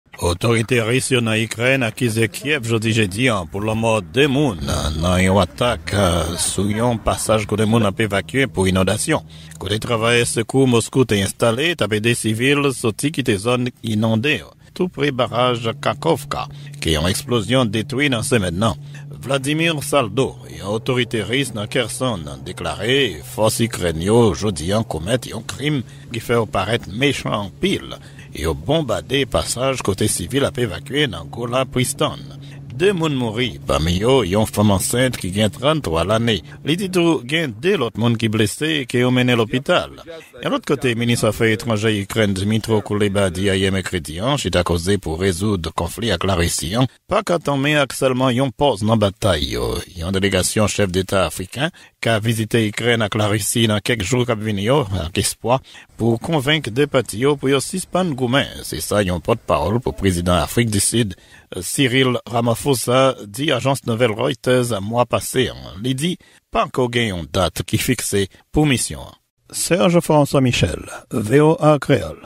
Larisi Akize Ikren Deske se Limenm ki Responsab Lanmo 2 Moun Pandan yon Evakyasyon
Larisi akize Ikren deske li responsab pou lanmo 2 moun ki mouri pandan yon atak sou yon operasyon evakyasyon.